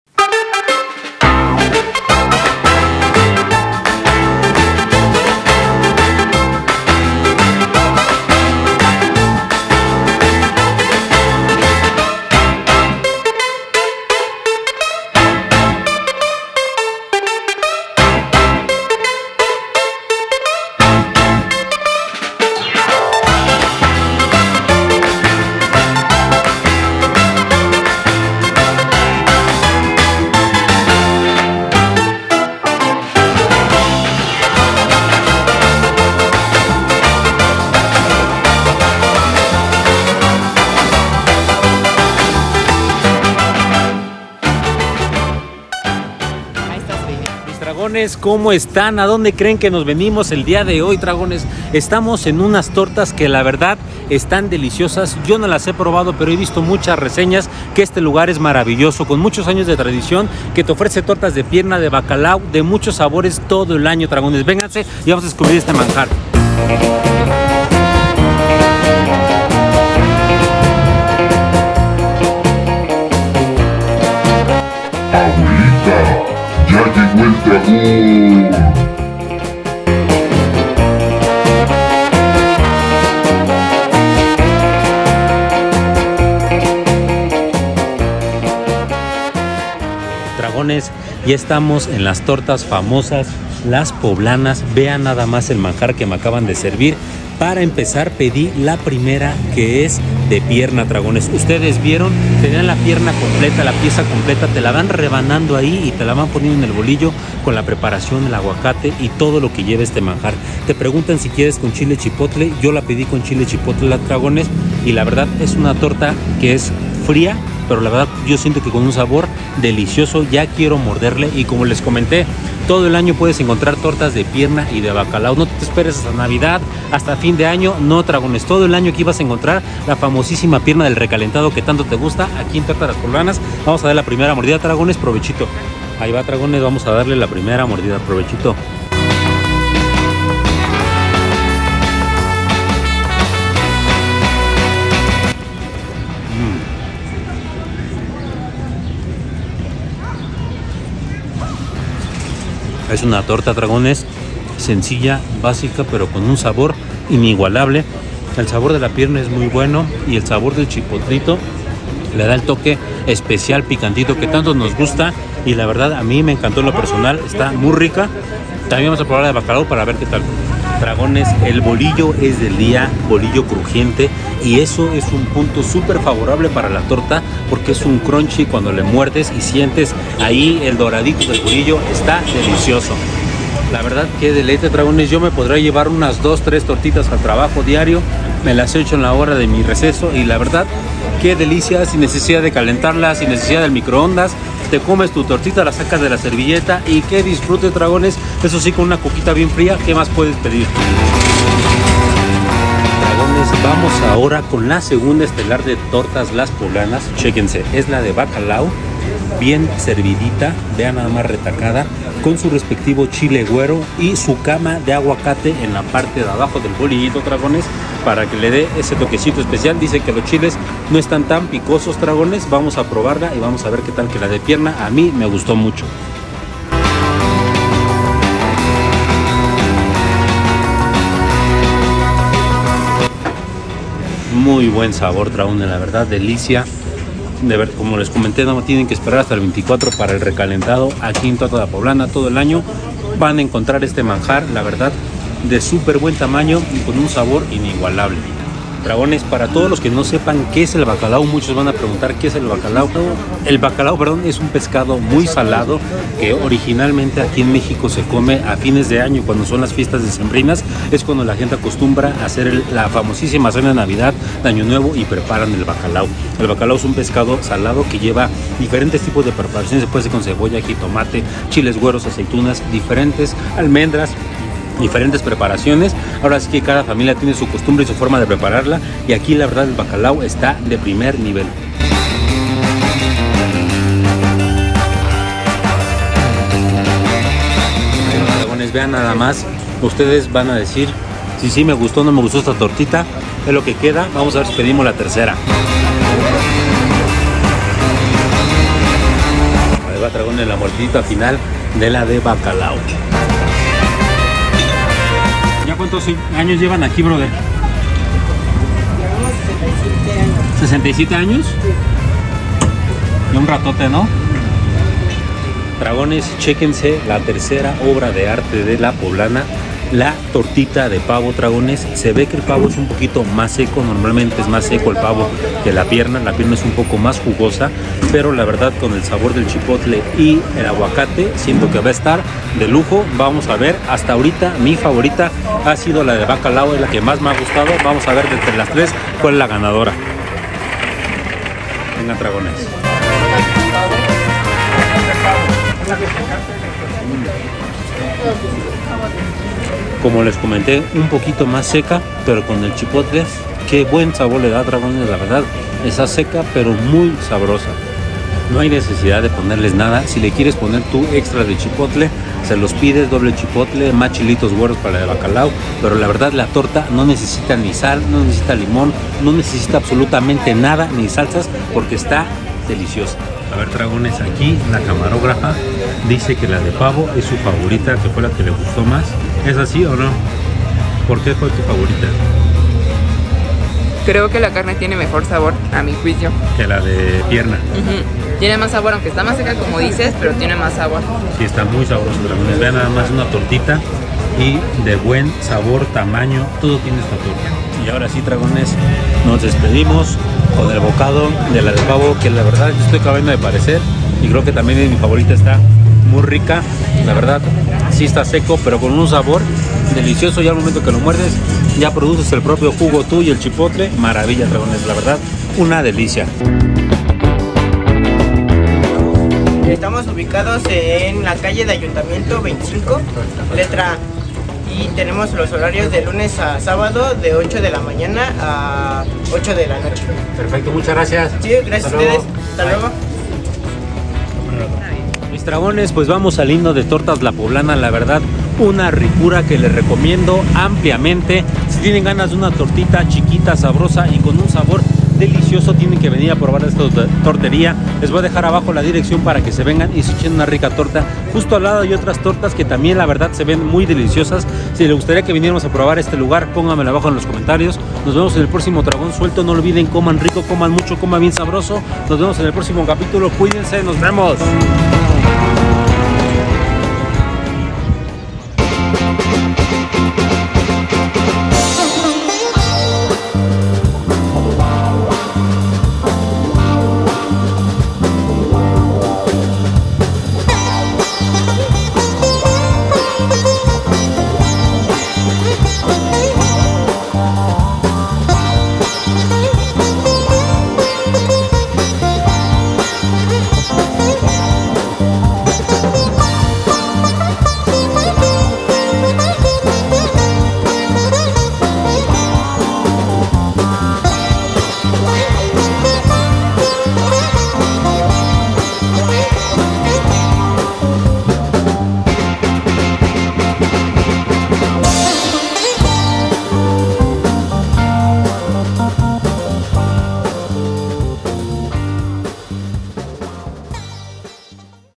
TORTAS CLÁSICAS de PAVO y BACALAO TODO el AÑO! 🦃
Unas TORTAS de PAVO, PIERNA Y BACALAO todo el año en el CENTRO de la CDMX, las famosas TORTAS "LAS POBLANAS", ubicadas en:

Ayuntamiento 25A, Colonia Centro, Centro, Cuauhtémoc
C.P. 06000 Ciudad de México